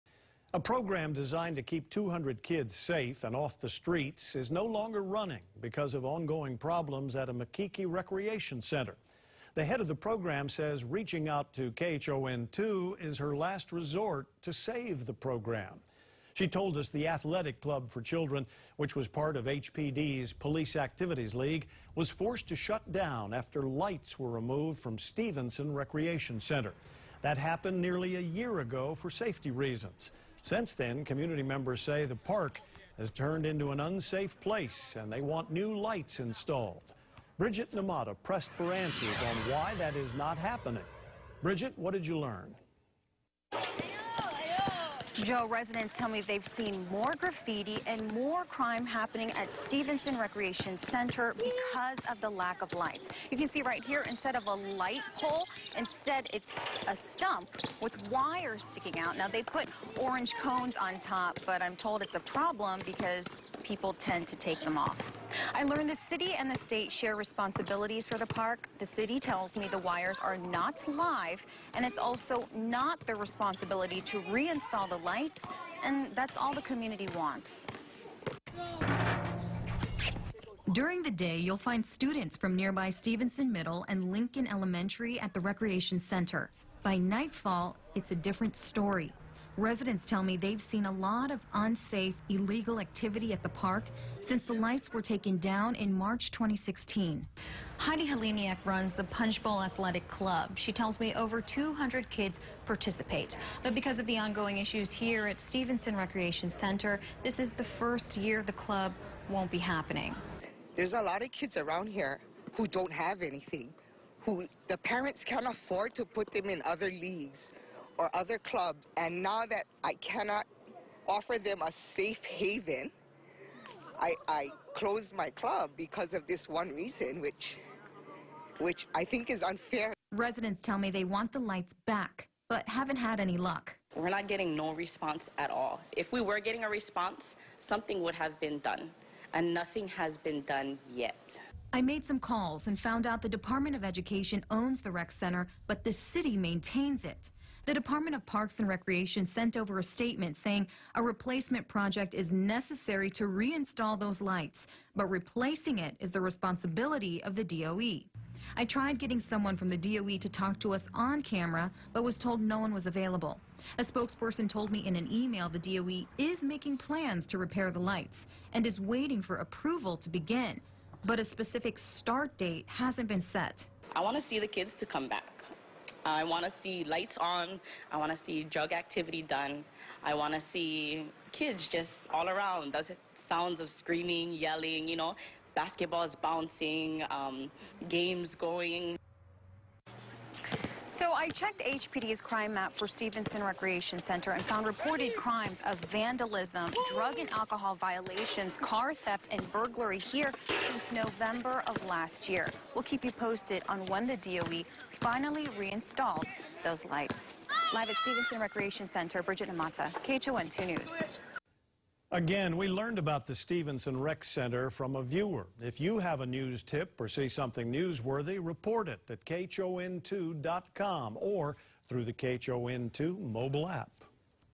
Recreation center's ongoing safety issues jeopardize community keiki program
The head of the program says reaching out to KHON2 is her last resort to save it.